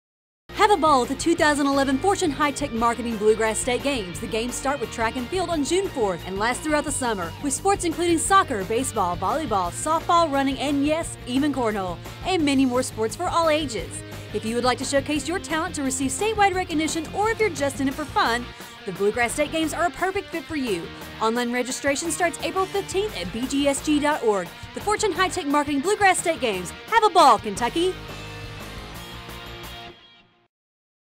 FHTM Bluegrass State Games
FHTM is excited to once again be the title sponsor for the Bluegrass State Games.  It's a great tradition that supports athletes throughout Kentucky.  Thanks to WKYT for this video.